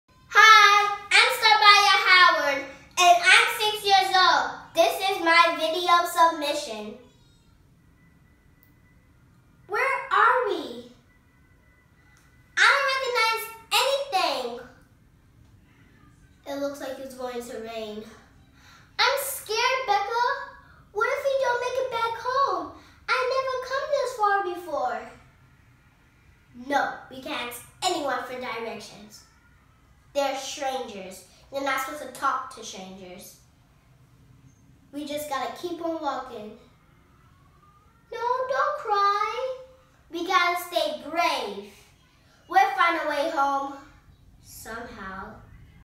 Child Monologue - Starmya Howard
Hey guys! I am doing a video submission saying children monologue.